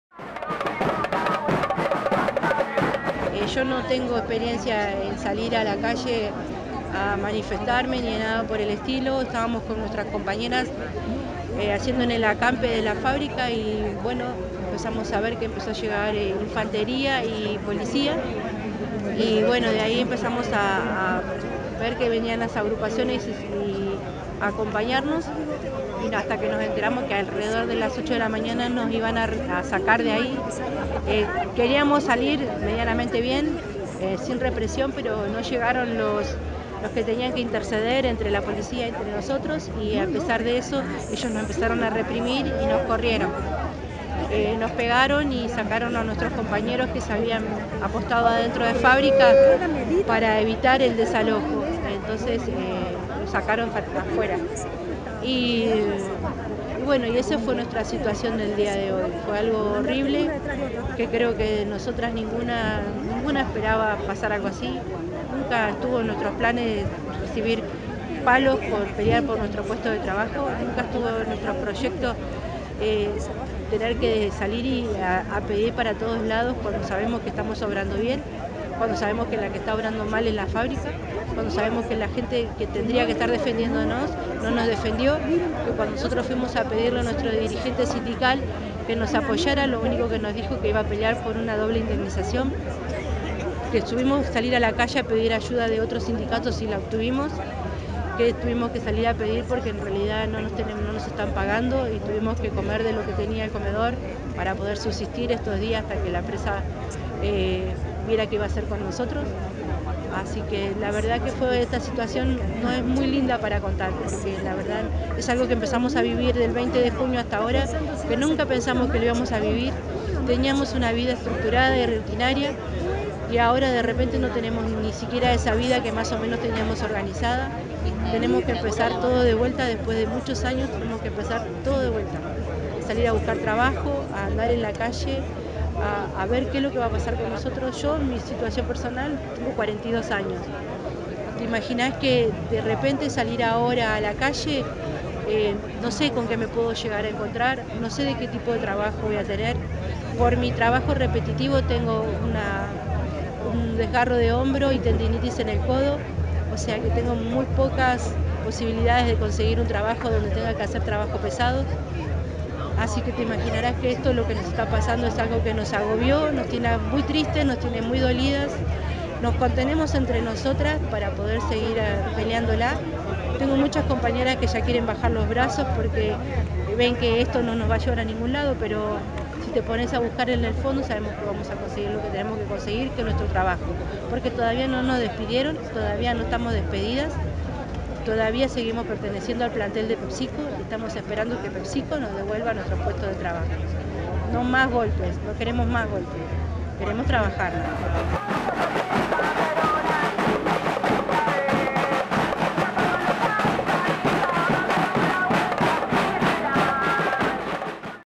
Entrevista a Delegada Pepsico
Luego de la represión en la planta de Pepsico,  la Delegada de la Comision interna de los trabajadores de Pepsico Patricia Mora narra la situación.